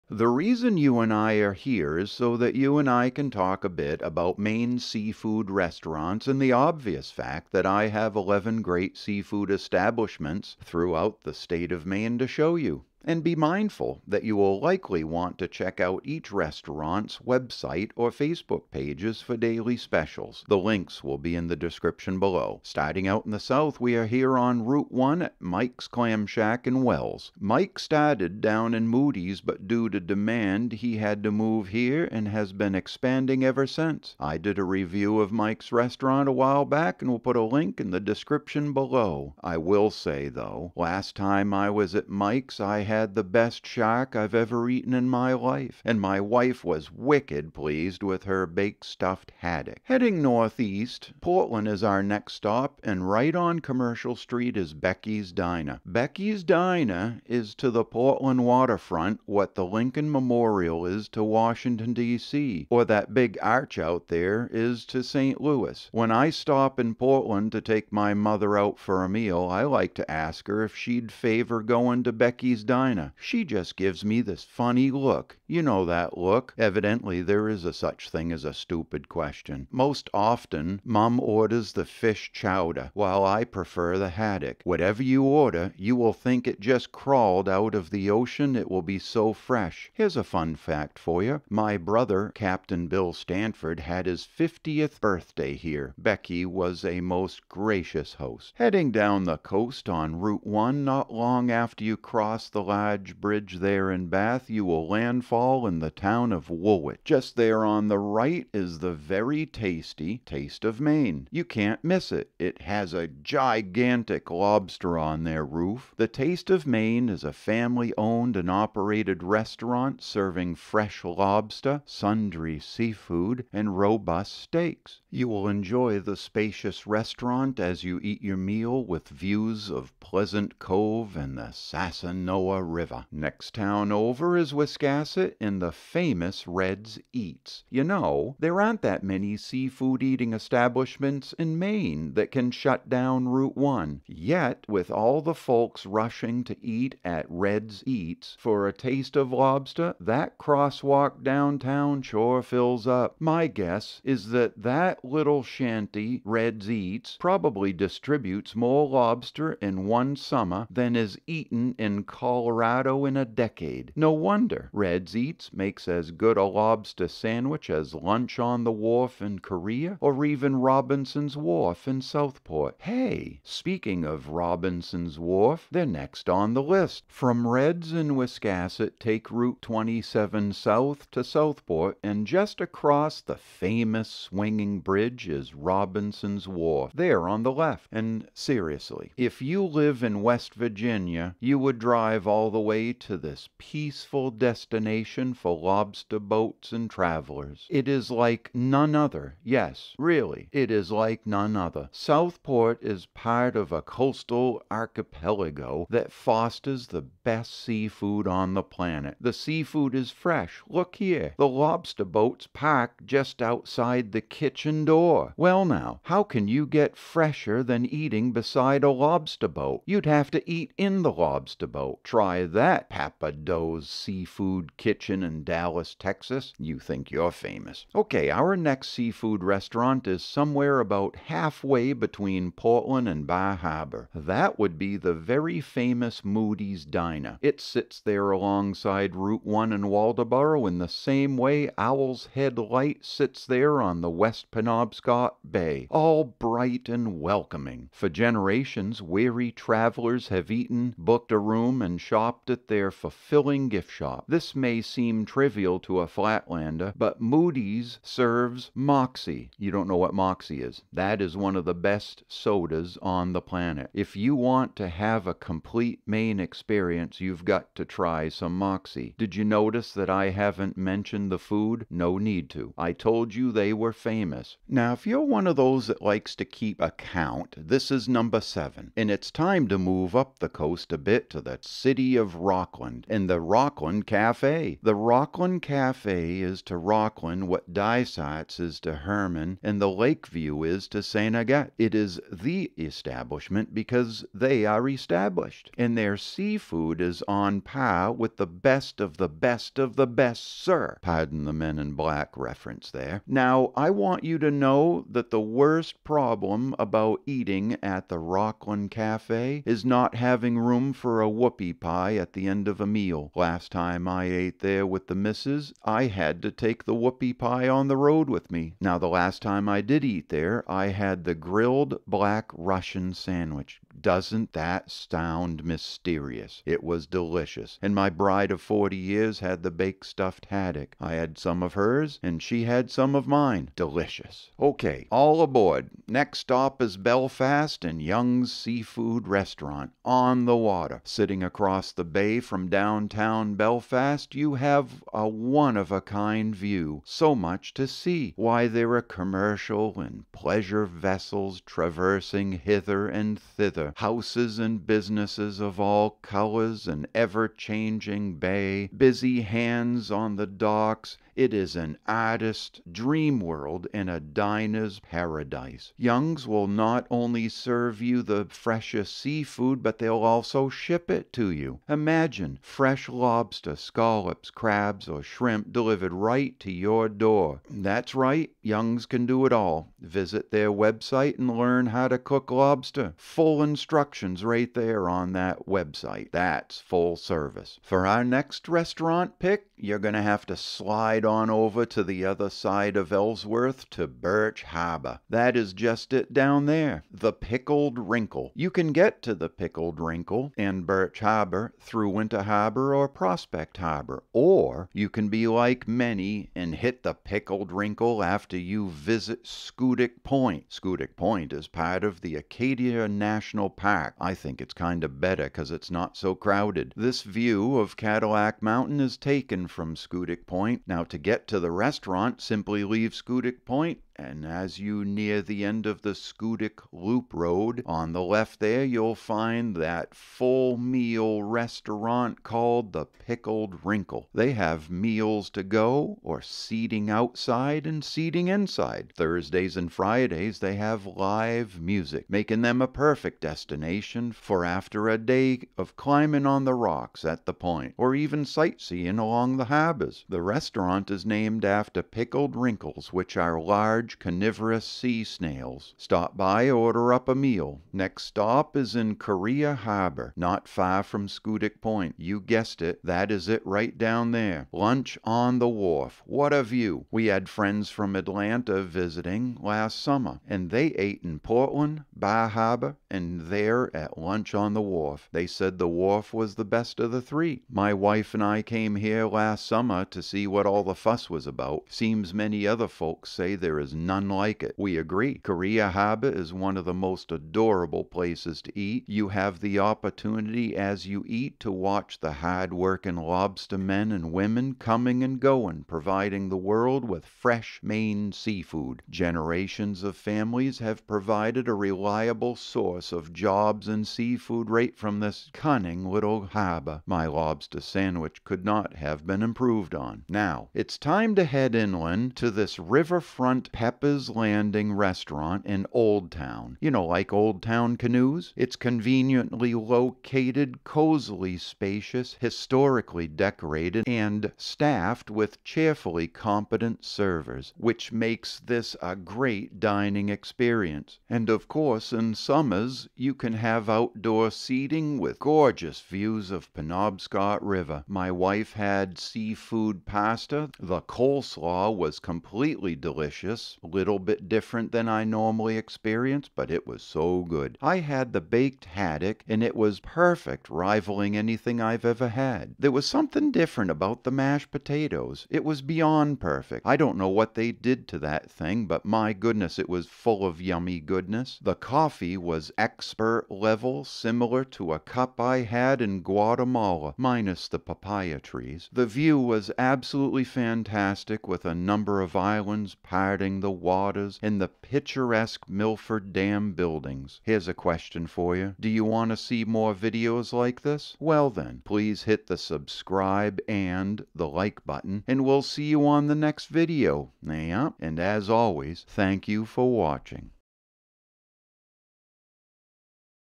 Eating Seafood in Maine | 2023 Updated List
Here are Eleven GREAT Seafood Restaurants in Maine. Established. Proven. Tasty.
Gifts of Maine:
Maine Whoopie Pie
Maine T-shirt
Maine Socks
Lighthouses of Maine Tapestry
Maine-iac T-shirt
Maine is Calling T-shirt
Hiking Maine Guide
Maine Brass Christmas Ornament